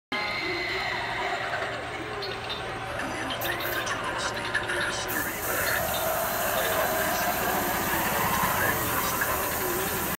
halloweenattgevillagesflclip1
Halloween Decorations at The Villages, FL Clip 1

The Villages Florida is a community with the best Halloween decorations I have ever seen . This house had professional quality decorations that attracted an audience